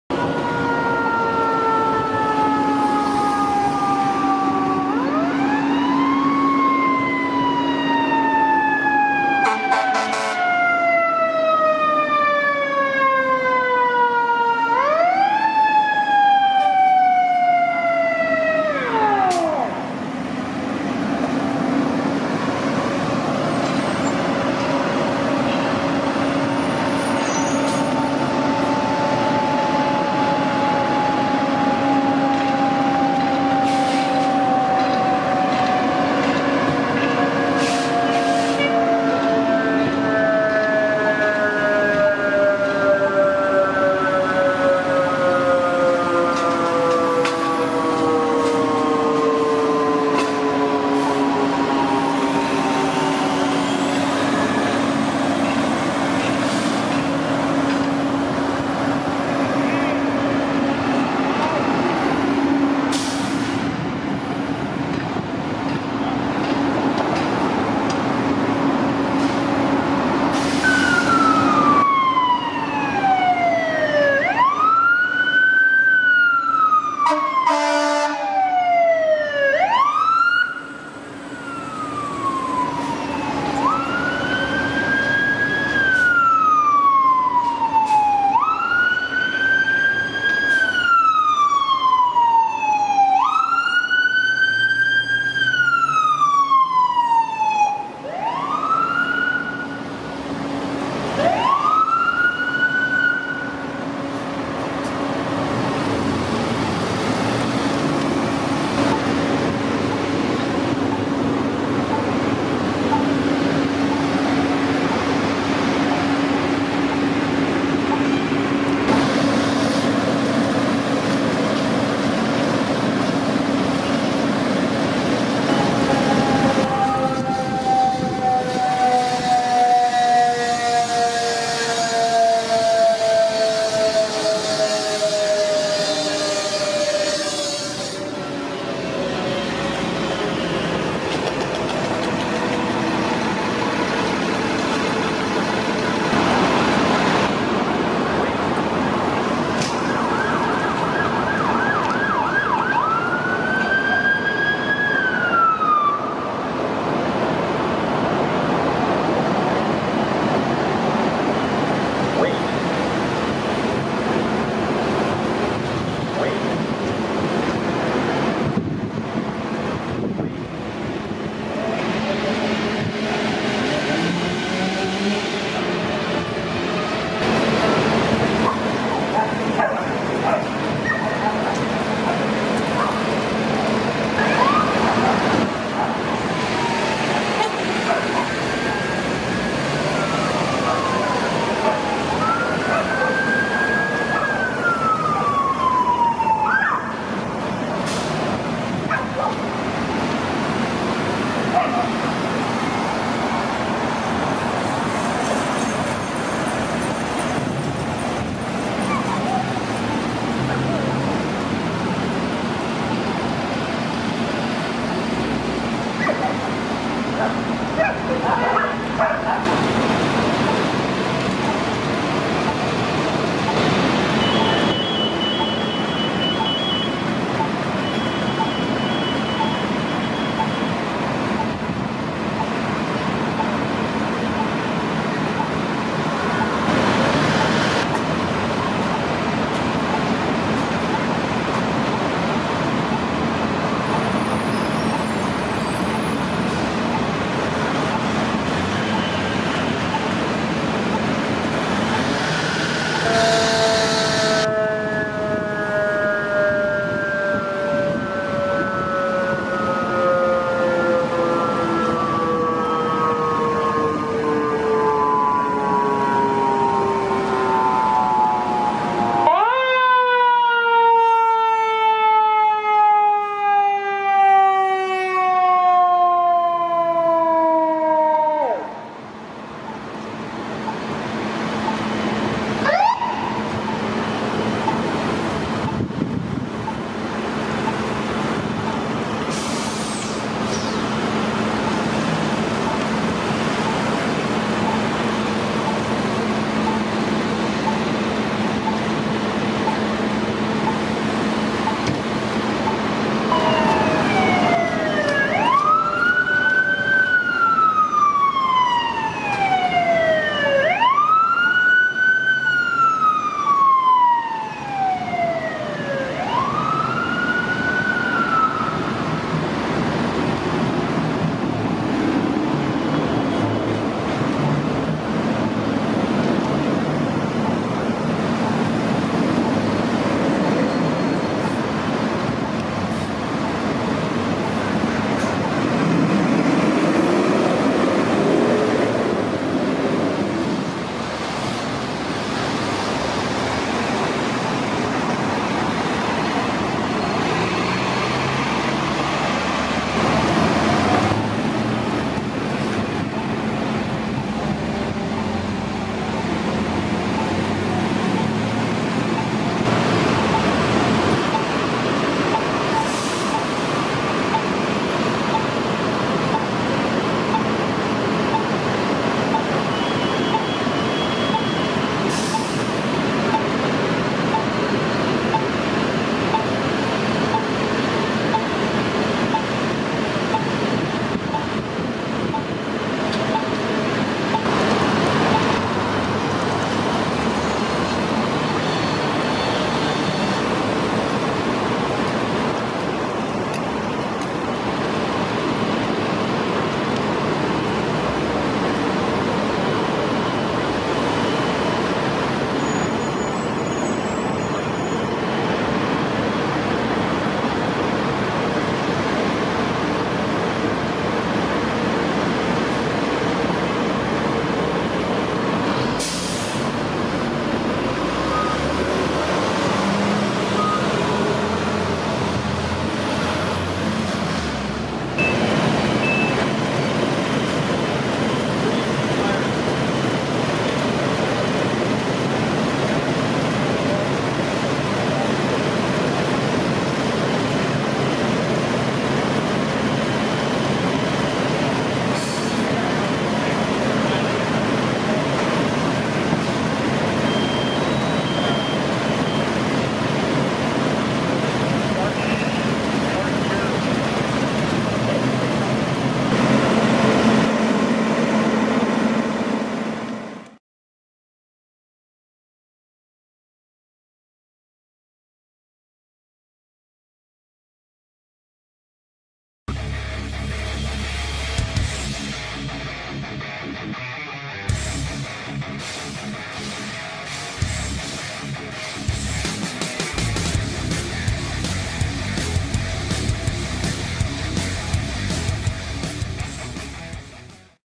*Huge* Seattle Fire response: E18 L8 E21 B6 M31 L9 A86 M44 Saft2 Dep1 Staf10 E20 Rehab1 L10 E9
I was still in bed at 8:03 this morning (April 2) when my PulsePoint app went off.  I saw where the call was, threw on some clothes, started recording the traffic camera on my laptop, then jogged up to 15th Ave where I just missed Battalion 4 arriving, but managed to catch Engine 18 pulling up just as a tree behind the building burst into flames.

I got lots of great responses, with just a few snaffus.  My camera died as Aid 86 was pulling up, and was slightly out of focus for the next shot.  A few of the units went around the block, so I missed getting a good shot of Ladder 10; I was also pretty disappointed that Aid 260 arrived without lights/siren.

Overall, though, can't complain about getting this many catches on a Saturday morning.  No fire details released yet, although a homeless woman on scene told me the fire had likely been started by people stealing copper wire inside the (derelict) building.  This is a row of abandoned buildings in Crown Hill (north Seattle); the fire was inside a closed bar called Goofy's.  No injuries reported.

So, I obviously need a new camera, and now that I have a proper job again I can afford one.  Any video camera suggestions, or models to avoid?

Clip listing:
0:00 – E18 arriving, while tree bursts into flames
0:41 – L8 arriving
1:12 – E21 and B6 arriving
1:49 – M31 arriving
1:59 – Brief shot of fire building
2:07 – L9 arriving with A86
2:26 – SPD, M44 and Saft2 arriving
3:05 – Dep1 and Staf10 arriving
3:40 - On-scene footage
4:14 – E20 arriving
4:44 – Rehab1 arriving
5:03 – L10 and E9 arriving
5:51 – Parking patrol arriving, and Seattle City Light cutting off power
6:25 – Air 260 arriving
7:06 – On-scene footage, and a few photos
7:50 - Outro